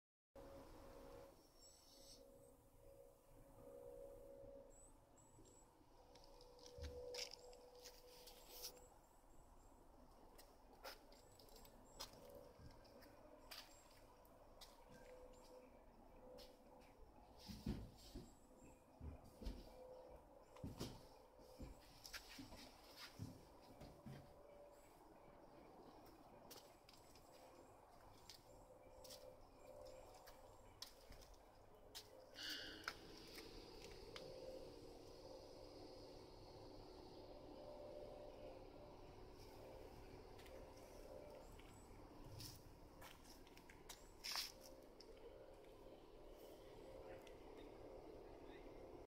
CroftgateUSA beading from Quick-N-Slick
Walk around showing some morning beading thanks to CroftgateUSA Quick-N-Slick